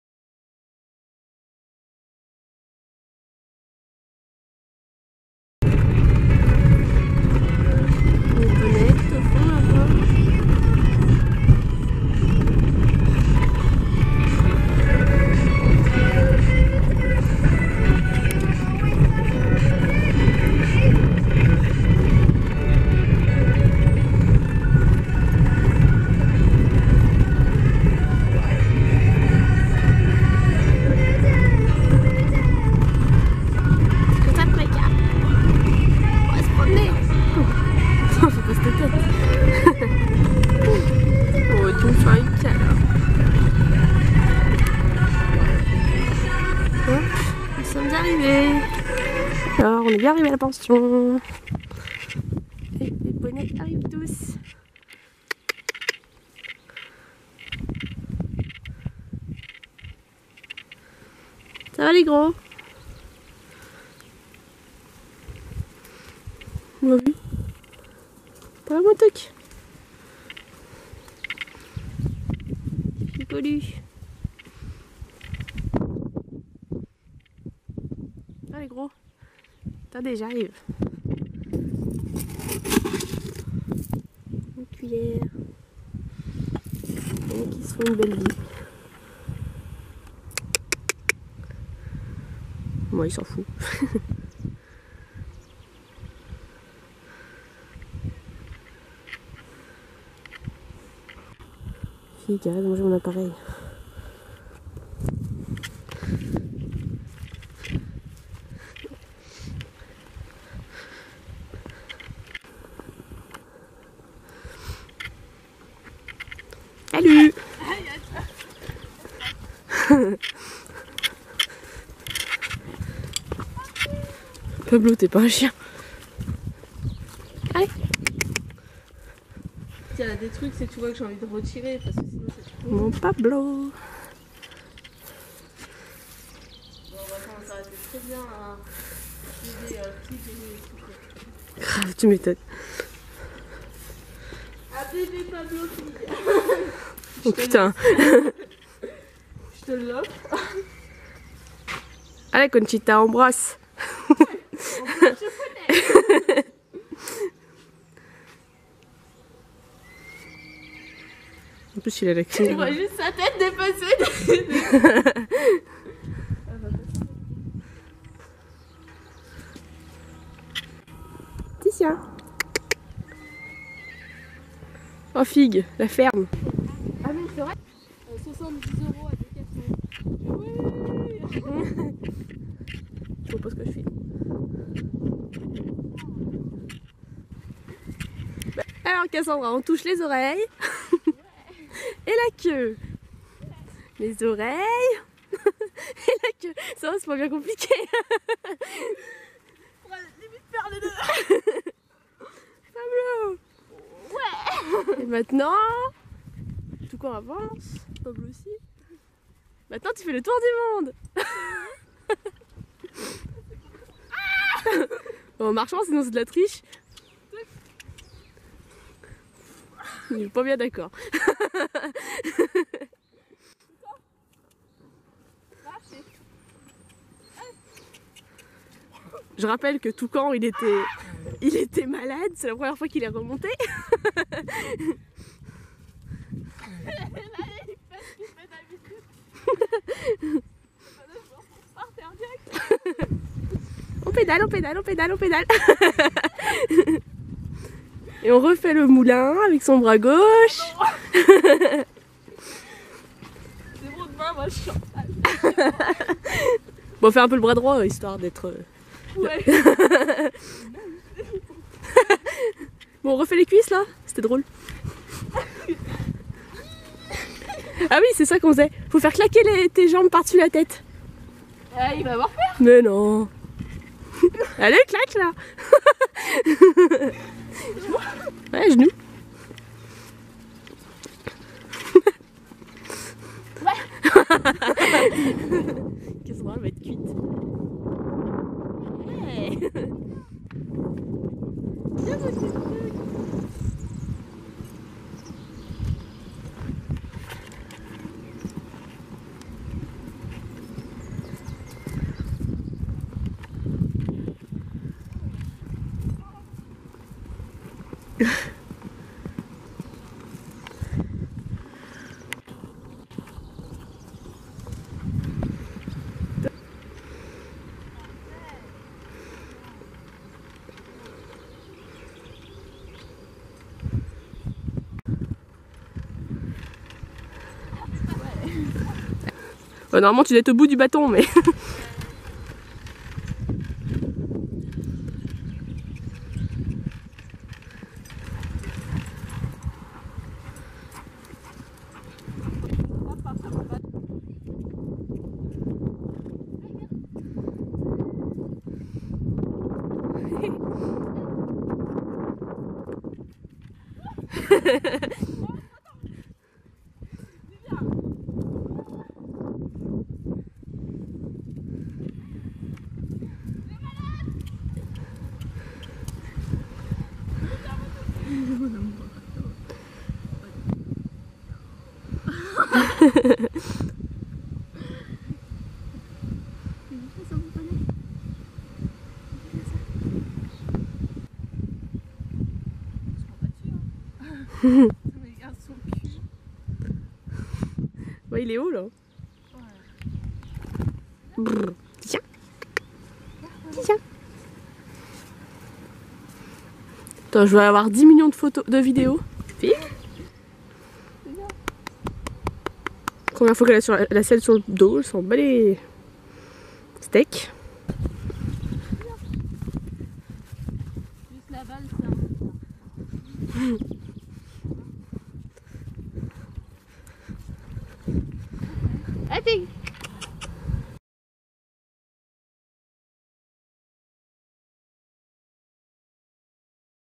15 mai 2016, un bout d'après midi avec nous
Remise au travail de Toucan
Petite séance avec Figue en carrière